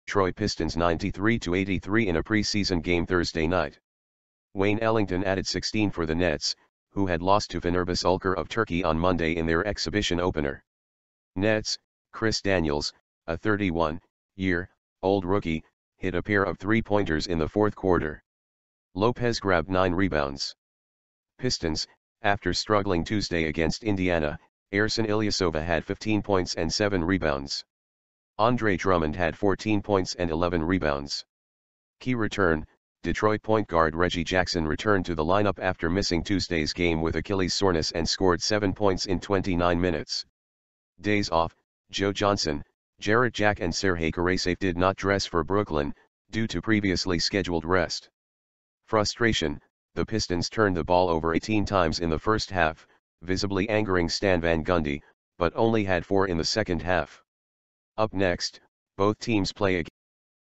NBA Recap Brooklyn Nets vs Detroit Pistons | October 8, 2015 | Highlights
Brook Lopez and Shane Larkin each scored 17 points and the Brooklyn Nets beat the Detroit Pistons 93-83 in a preseason game Thursday night. Wayne .